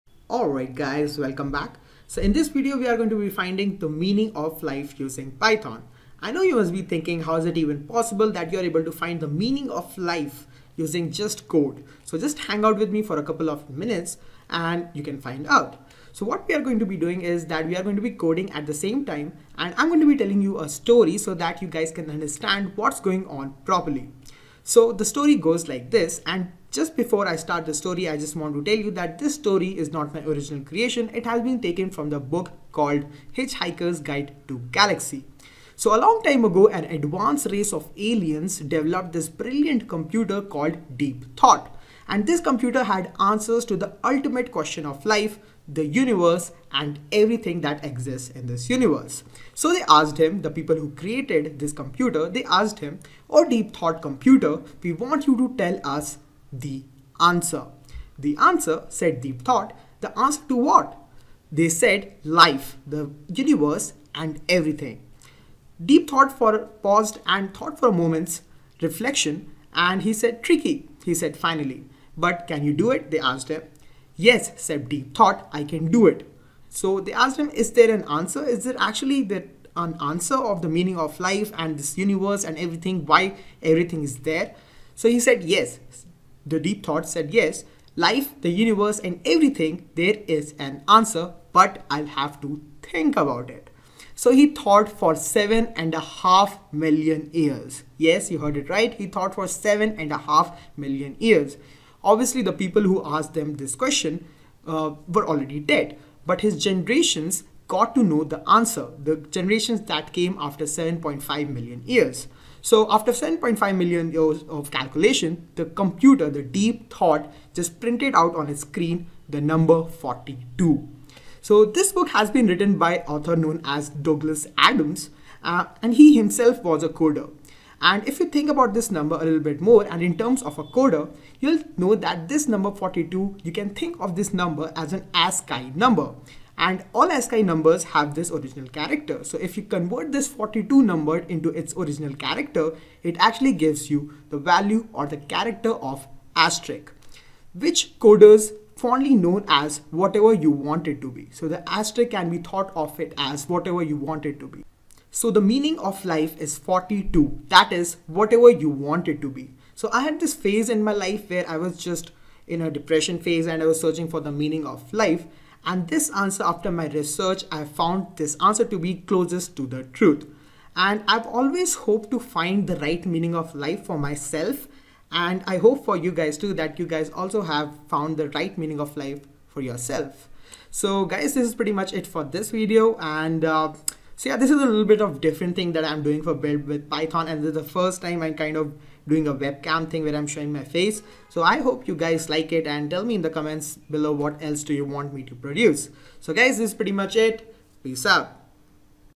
Finding the Meaning of Life and Universe using Python
I had phase in my life in which I was searching for the meaning of life myself. And this answer is the closest to truth I have ever seen.

I have always hoped to find the right meaning, for my life and now I hope you do too...using Python